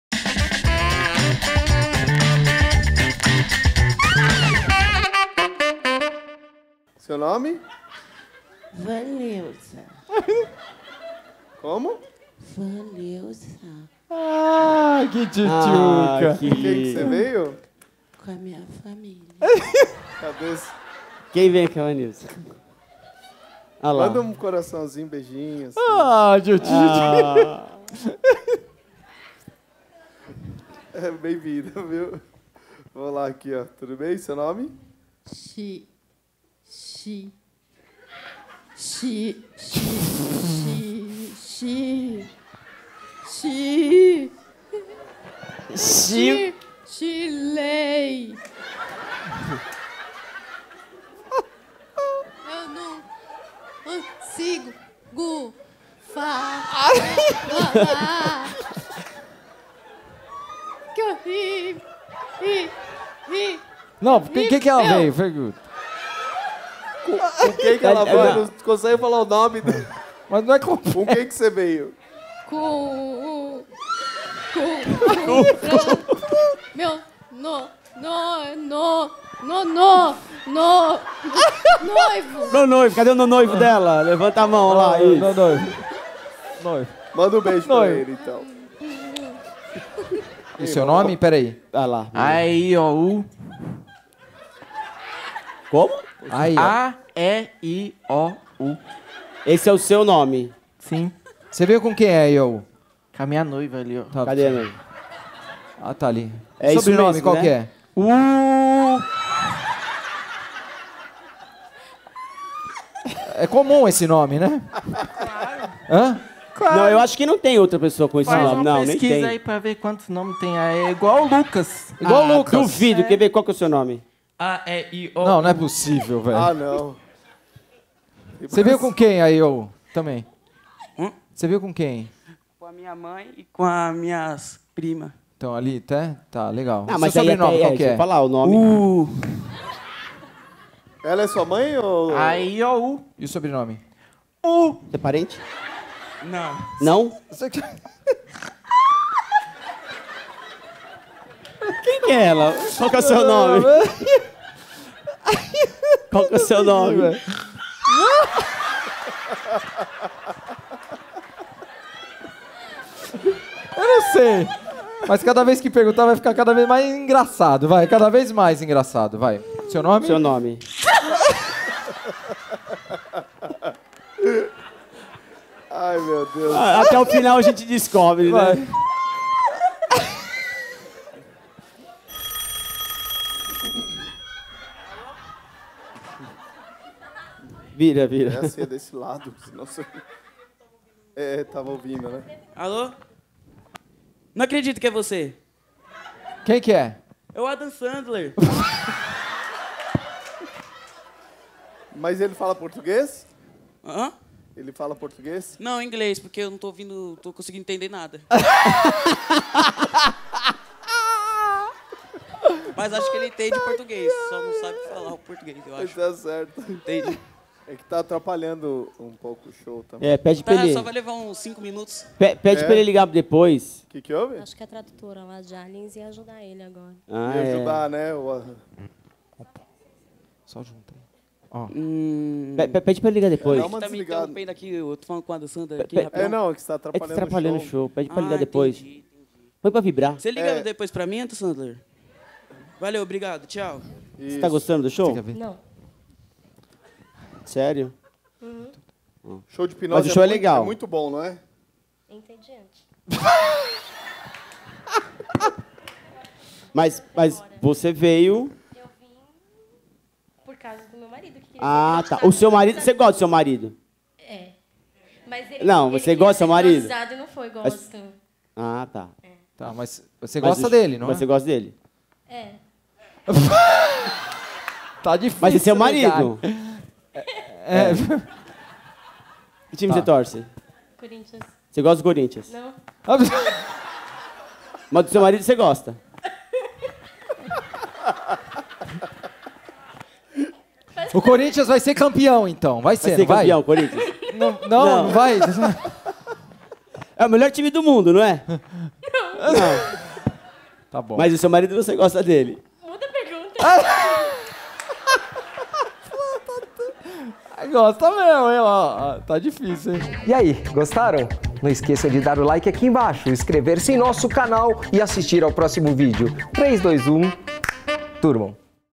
SHOW DE HIPNOSE CÔMICA - HIPNOTIZADOS NO PALCO E MUITA CONFUSÃO (11/11/17)
Em nossa apresentação do dia 11/11, muita confusão entre os hipnotizados no palco !! E risada geral !!

*** TEATRO FOLHA ***
Local: SHOPPING PÁTIO HIGIENÓPOLIS
2017 - Setembro à Dezembro - Todos Sábados às 23h59